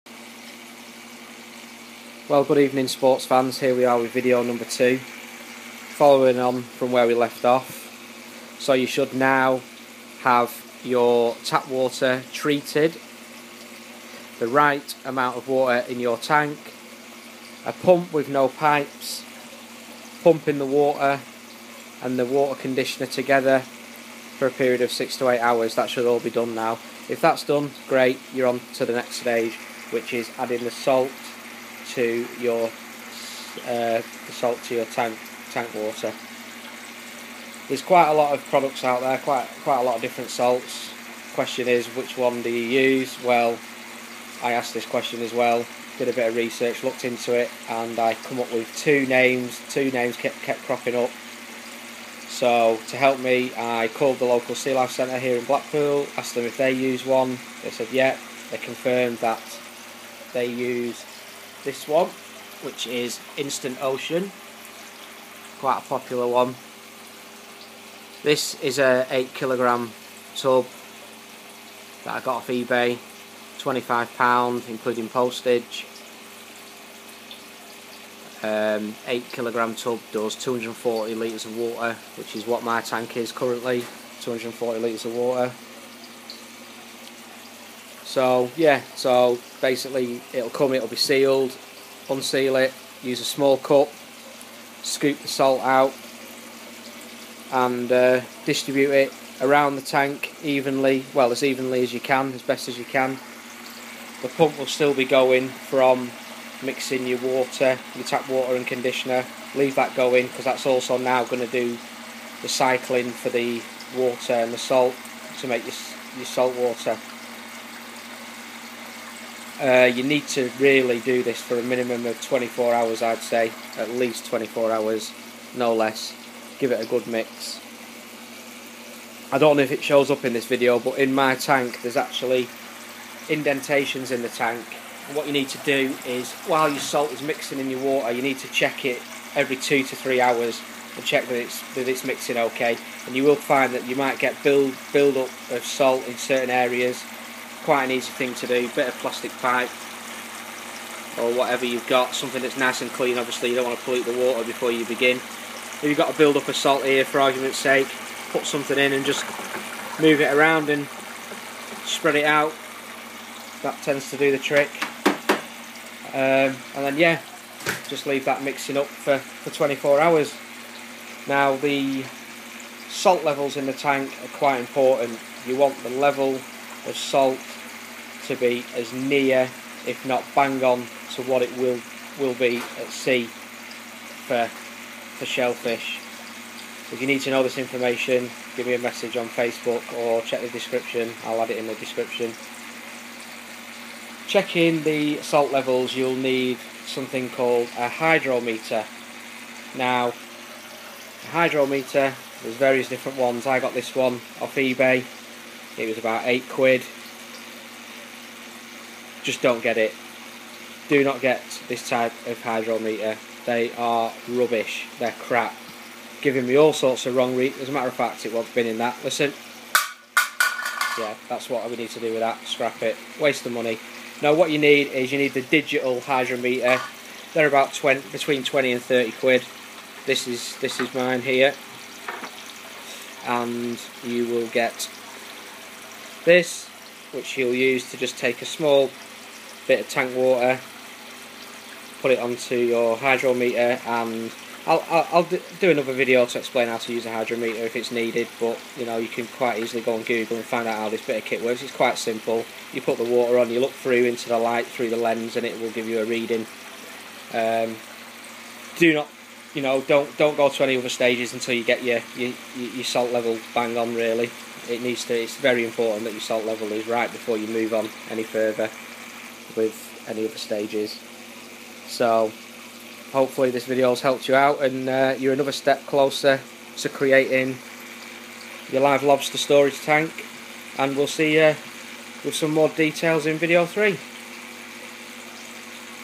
Live lobster storage tank - Video 2
Video 2: Adding marine salt

In this video I explain how to mix marine salt in to your conditioned water in your tank. I also give my opinion on the best salt to use. To confirm, the salt product used is called instant ocean. I bought an 8kg tub off ebay for £25 including postage. Same goes for information on the correct reading of salt level (salinity) of your salt water. It is important to get your salt water reading at a steady rate and a rate that is as close to what lobster live in out at sea.